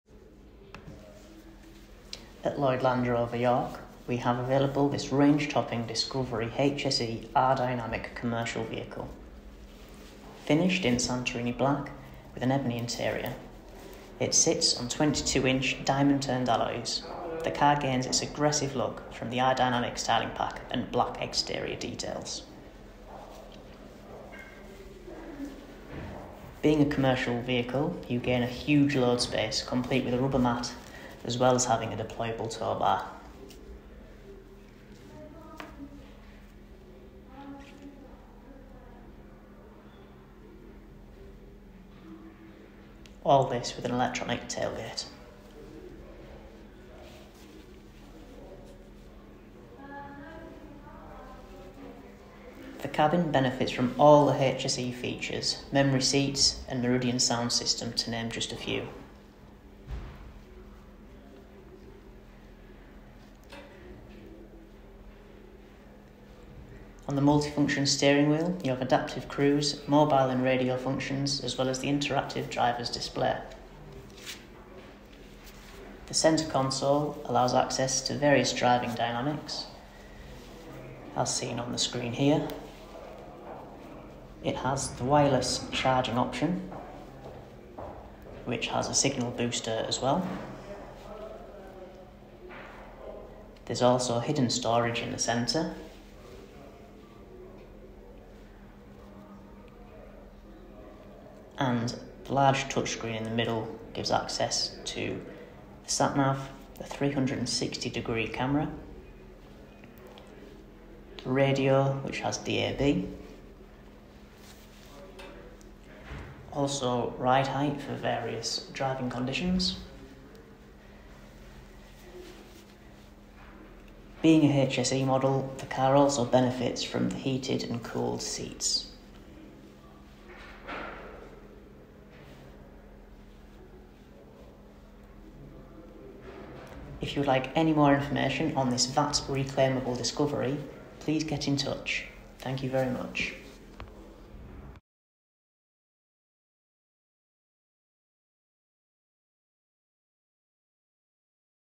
LAND ROVER COMMERCIAL DISCOVERY 3.0 D300 R-Dynamic HSE Commercial Auto
** VAT RECLAIMABLE ~ FLAT BED ~ 22" ALLOYS ~ ELECTRIC TOW BAR ~ HEATED/COOLED SEATS ~ ADAPTIVE CRUISE** Here at Lloyd Land Rover in York, we are pleased to bring up for sale, this stunning Discovery 3.0 D300 HSE R-Dynamic Commercial, finished in Santorini Black with an Ebony Windsor Leather Upholstery. New in November 2022. Being a commercial vehicle, our Discovery is VAT RECLAIMABLE. Of course, being a HSE includes 11.4-Inch Touch Pro, Memory Seats, Meridian Sound System, Rear Camera with Park Assist ** Our discovery also includes a DEPLOYABLE TOW BAR, and Black Exterior Pack featuring Black Side Vents, Black Grille and Black Badges ** As with every Used and Approved Land Rover here in York, it carries our Strict 165 Multi-Point inspection and support from the retail team which means buying a Used Land Rover from us feels like NEW. Two features that helped us win What Car’s Best Approved Used Programme Award in 2018!